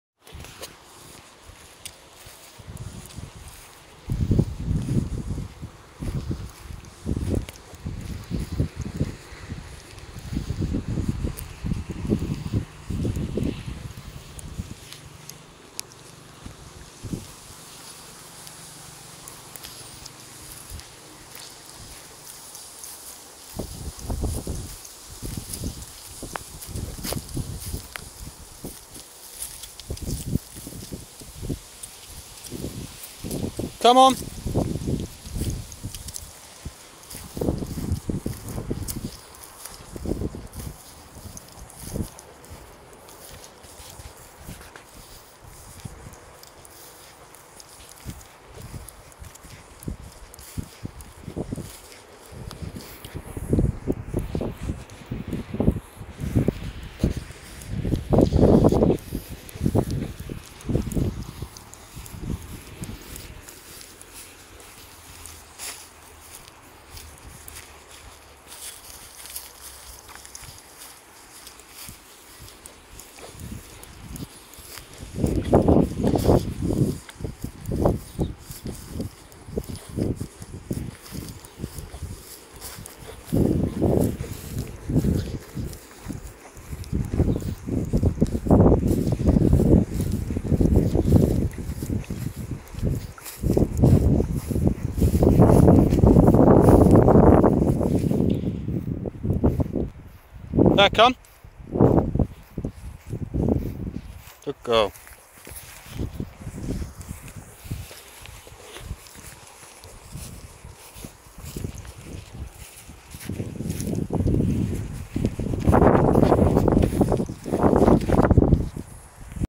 maltipoo puppy hiking
5 month old maltipoo walking off lead.